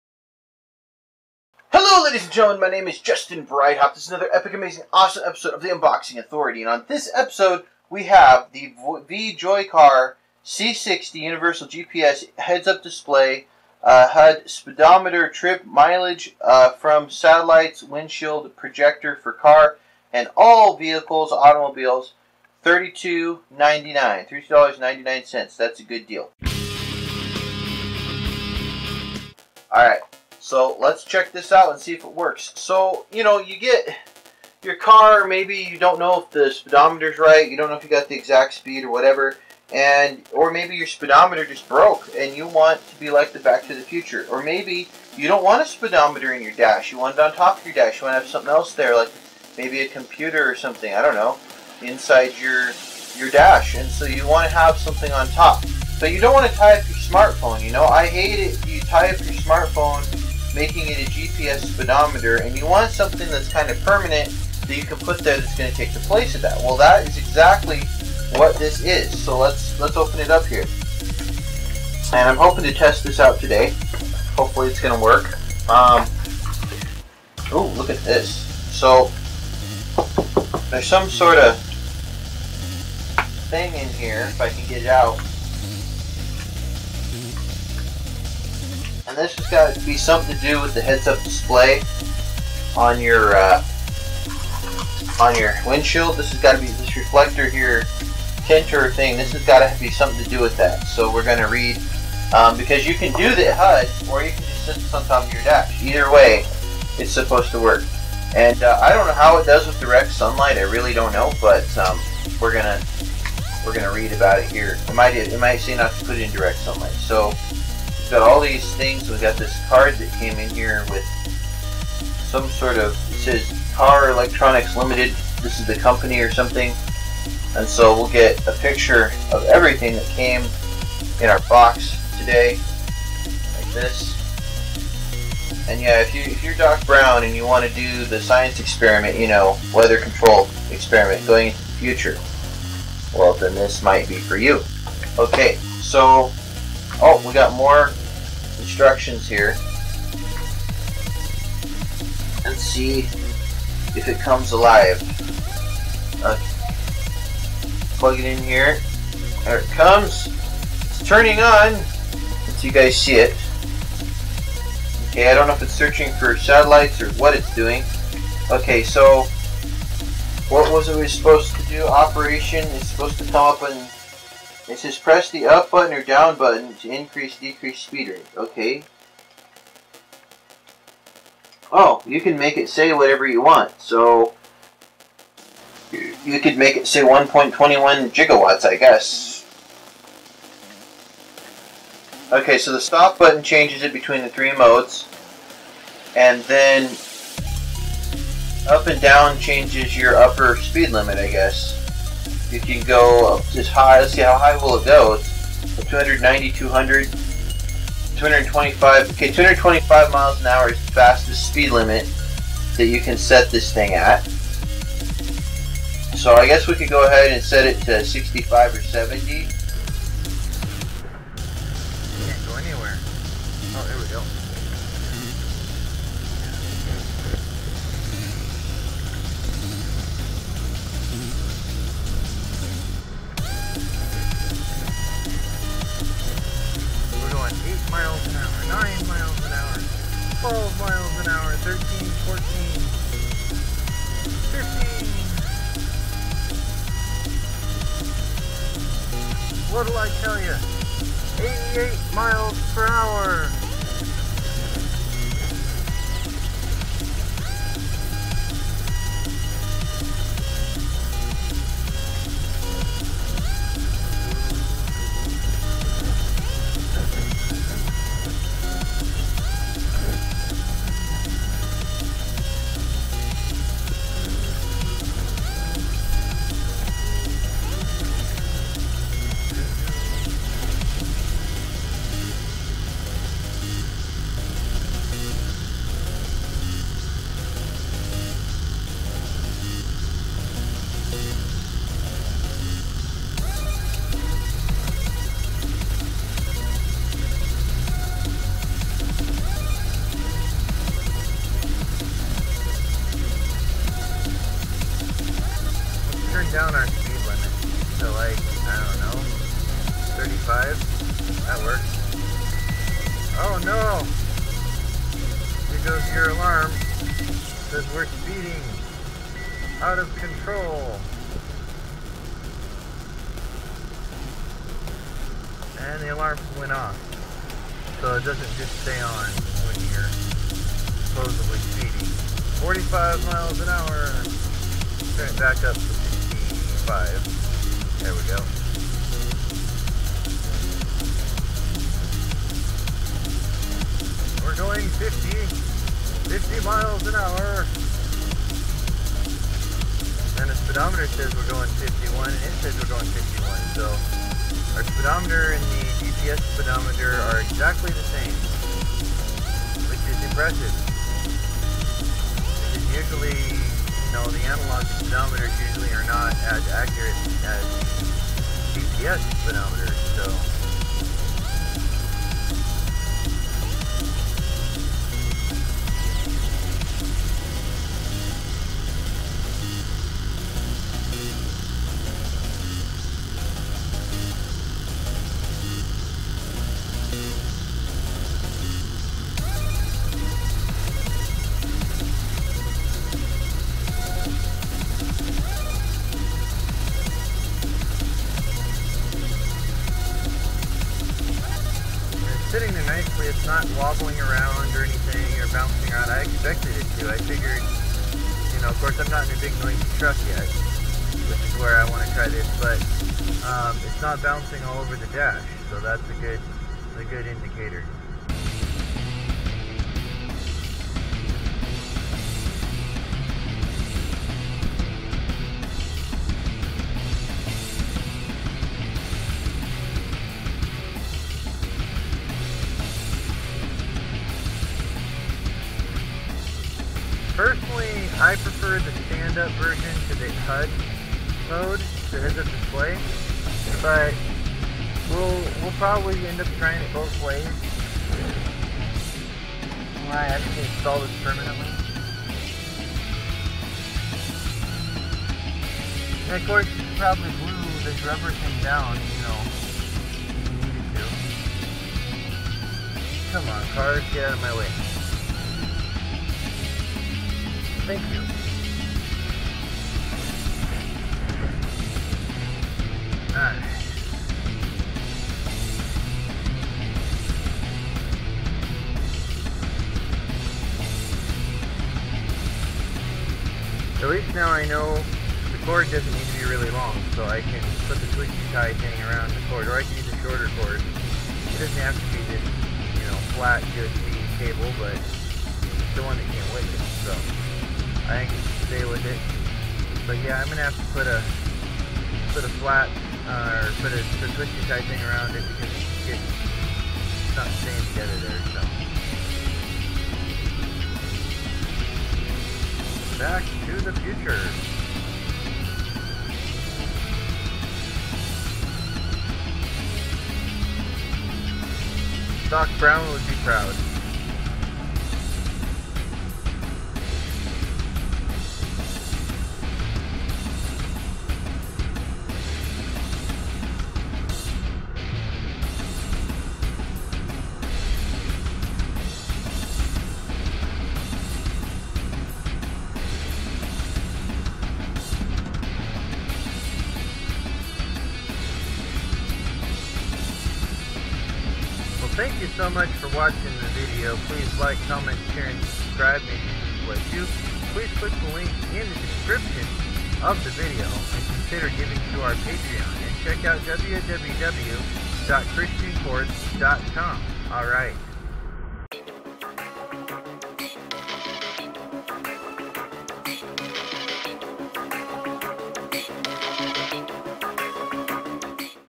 VJOYCAR C60 Universal GPS HUD Head UP Display Speedometer Trip Mileage From Satellites Windshield Pr
$32.99 This makes things so much simpler. I love the included reflector for your windshield, to make sure the HUD shows up. The controls are simple and easy to use. It does MPH or KPH. The rubber mount will hold the GPS HUD Speedometer vertical or horizontal. It comes with a very long, flat, USB power cord, so you can install the power cord however you want. It has an alarm for speeding. The display is very easy to read. It's plug and play.

#3 Get Firearm Insurance to Protect You In Court After You Defend Yourself and Others

YouTube Needs List
Justin's Wish List